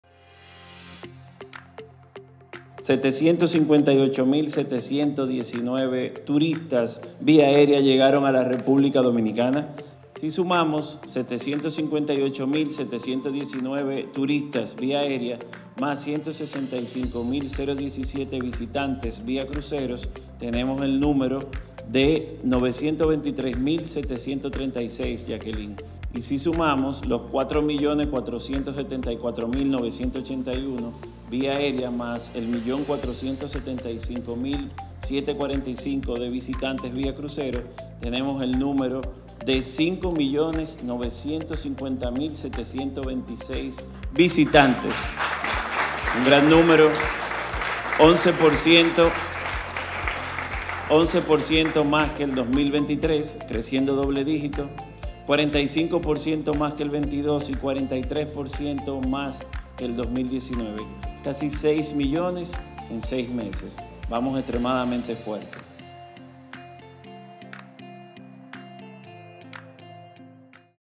El turismo llega a casi 6 millones de visitantes en solo 6 meses
Cerca de seis millones de visitantes llegaron a República Dominicana en el primer semestre de este año, récords que siguen marcando una racha de crecimiento constante del sector.